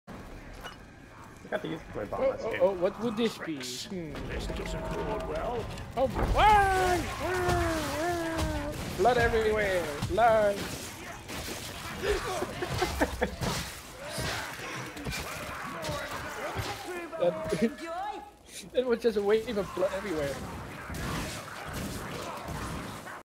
BLOOD AND FIRE!!
Master dwarf enjoys a little blood and gore here and there.

Sadly my voice was not recorded for this one, so just imagine I'm yelling with joy during the video.